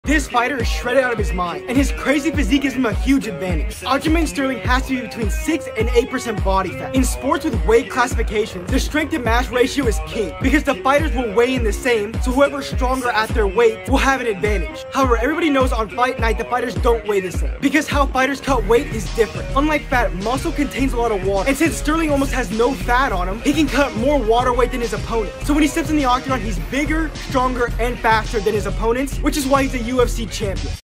The SHREDDED UFC Champion 🤯😈
In this video, we'll be taking a look at the SHREDDED UFC Champion Aljamain Sterling. Aljamain is a world-class fighter who has a shredded athletic body. His shredded body gives him a big fight advantage in the UFC and was on display at UFC 288 Aljamain Sterling vs Henry Cejudo.
If you're a fan of MMA or boxing, then you'll definitely want to watch this video! We'll be discussing Aljamain Sterling's crazy athletic body and how it helped him achieve such success in the sport of mixed martial arts. And of course, we'll be talking about his athletic aesthetic physique, which has helped him maintain a shredded physique throughout his career and helped him become a UFC Champion